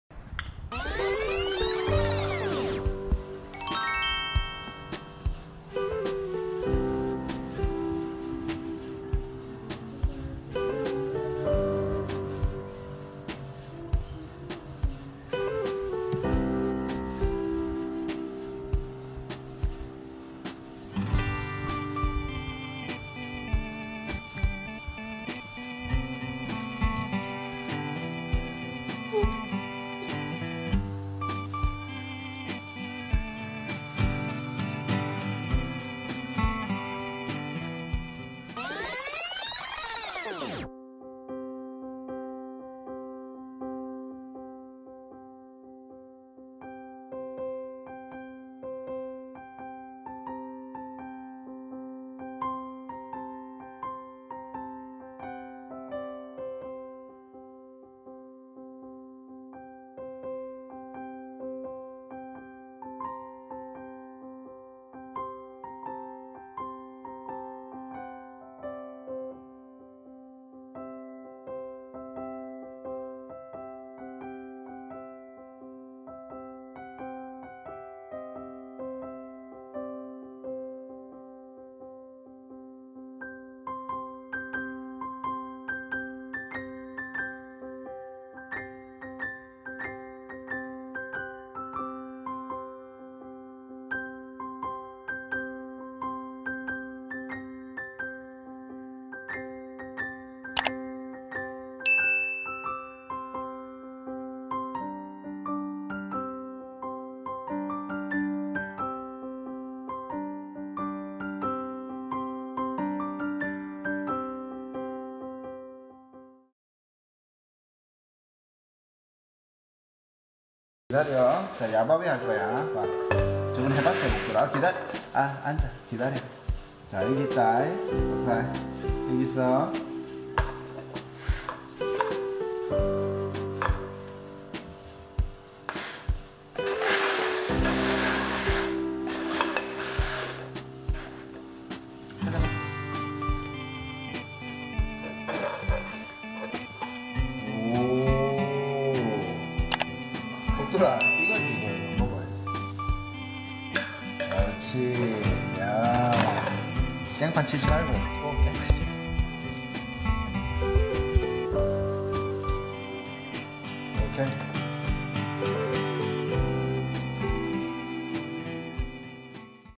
오스트랄리안 셰퍼드 2개월 강아지 4개월로 점프 했어요 ^^ Puppy change from 2 months to 4 months (오스트렐리안 셰퍼드, 오스트레일리안 셰퍼드)
복돌이가 처음왔을때 영상을 우연히 발견, 귀여워서 현재 모습이랑 비교하는 영상 올려요.
I found a video taken when Bokdol first came to our home. And compare it with the current one.